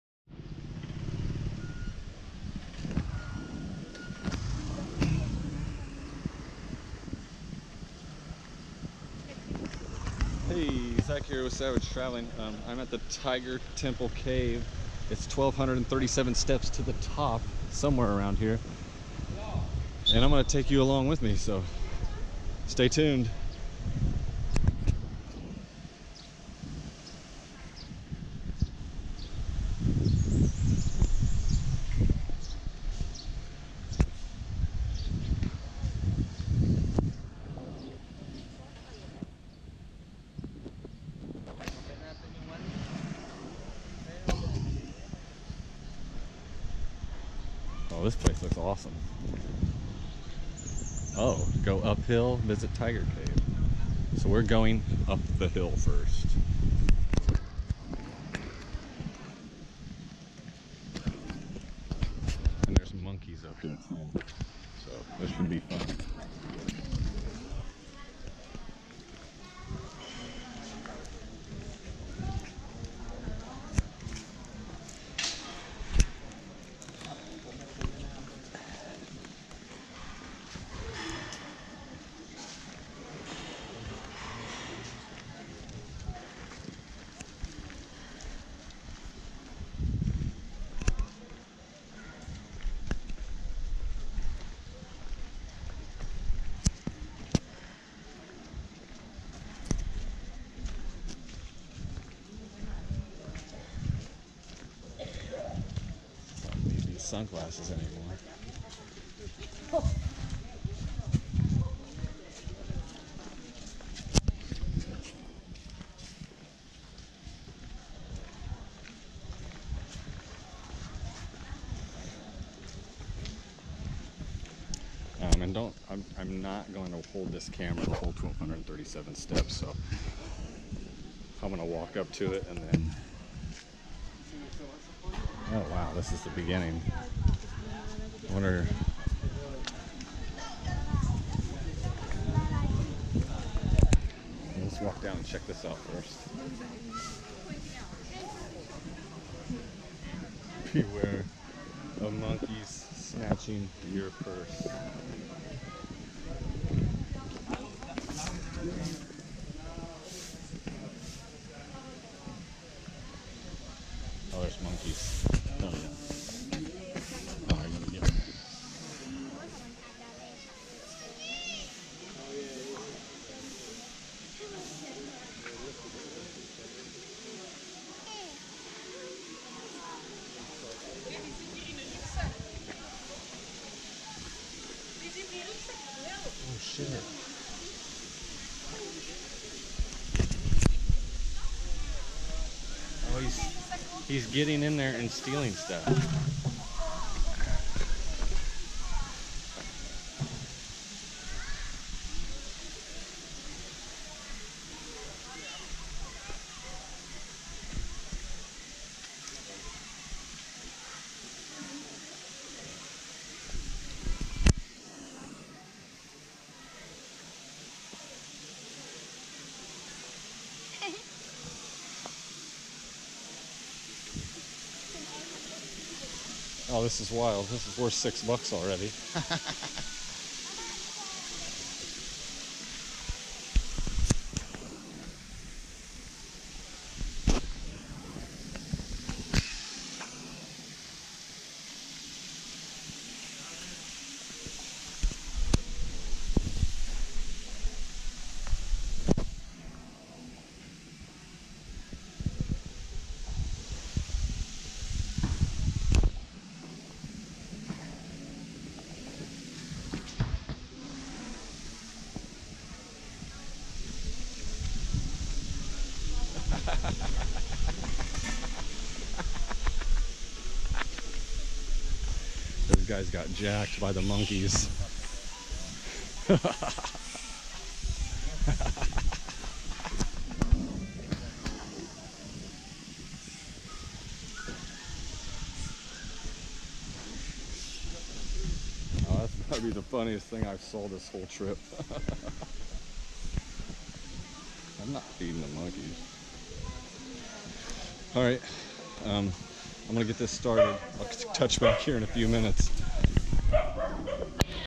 First Time At Tiger Cave Temple And I Catch A Monkey Stealing From Tourist!! Beware Of Monkeys!!!
Went to climb the 1237 steps to the top of the Temple and before i could get started I caught a monkey "red handed" stealing from a tourist bag!!! These tourist clearly didn't ready the sign posted at the from of the property. It was great time watching the monkeys and climbing to the top of the mountain.

CANON SL2 DLSR
GOPRO HERO 6
JOBY GORILLA POD
DJI MAVIC AIR FLY MORE COMBO
EMOTION STEALTH ANGLER FISHING KAYAK
INNO INA445 LOCKING KAYAK RACK